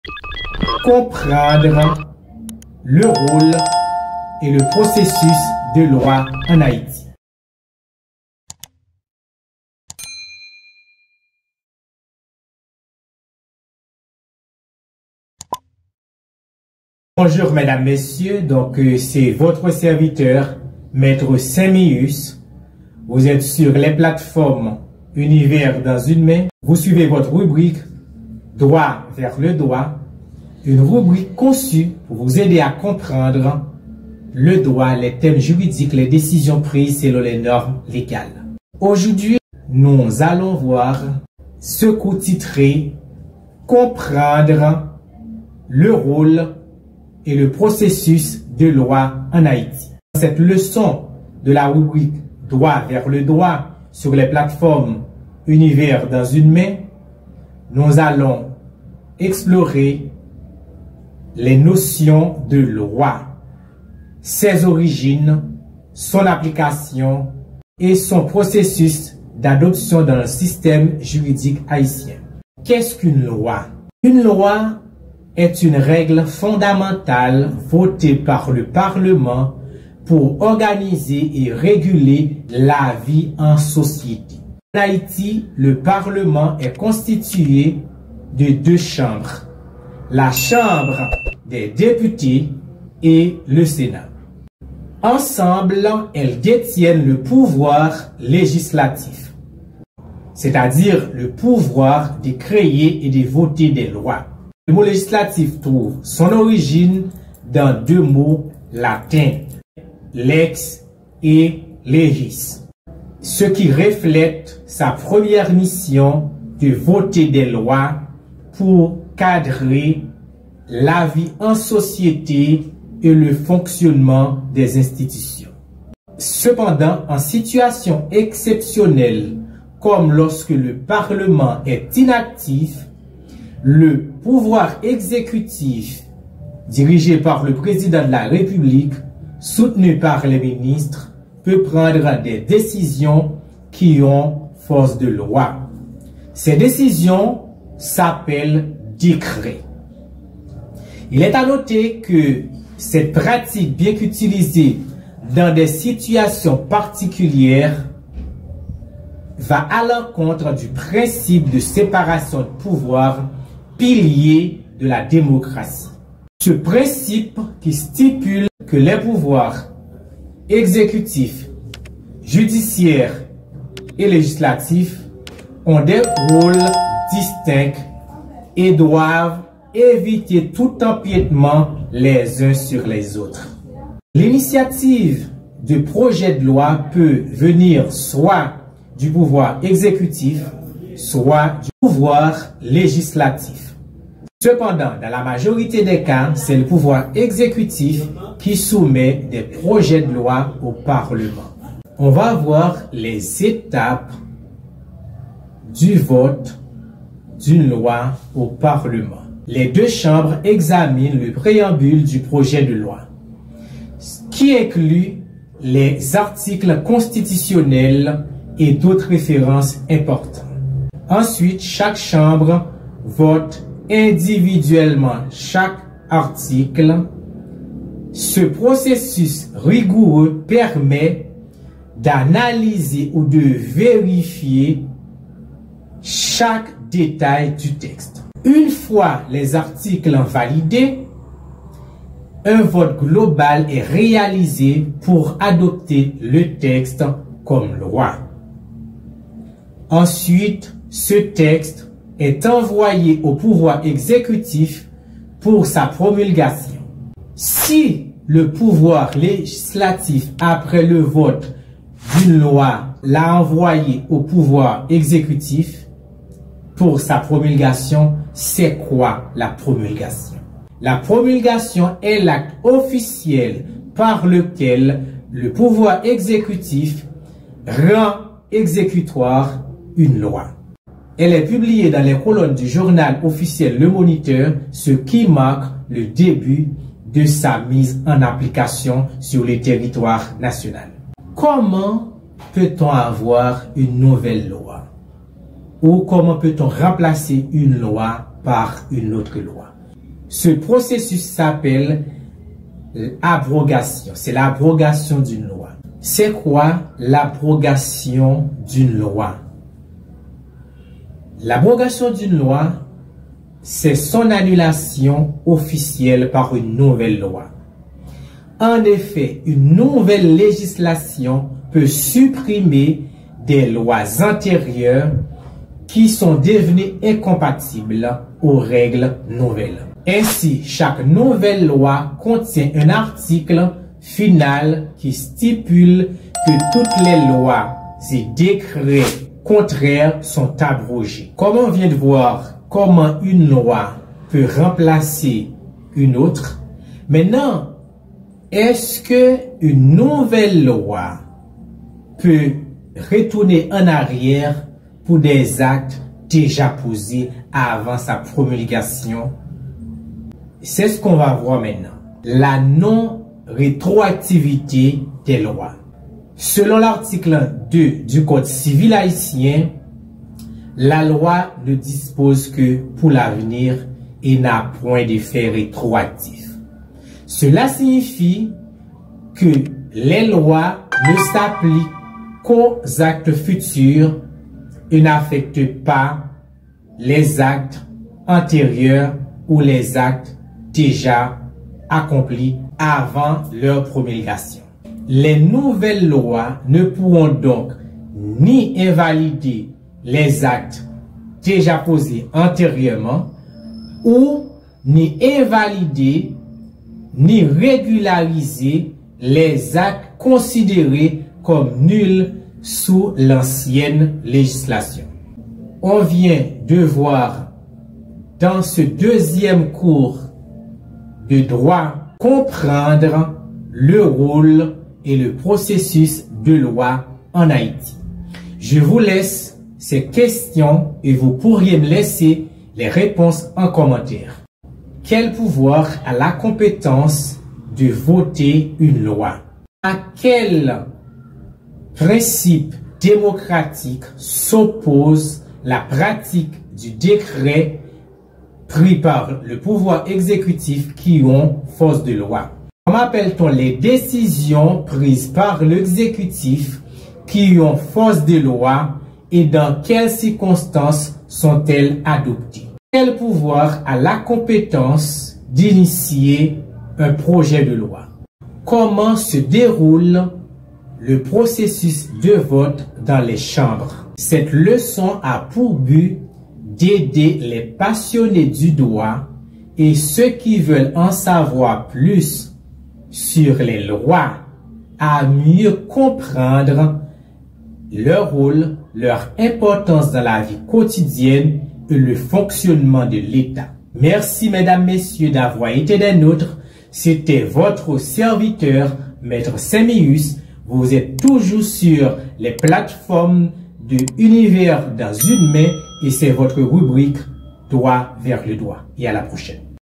Comprendre le rôle et le processus de Loi en Haiti.
1- Qu'est-ce qu'une Loi ?
2- Quel est le processus de vote au parlement ?

- Leçon 2
- Droit Vers le Droit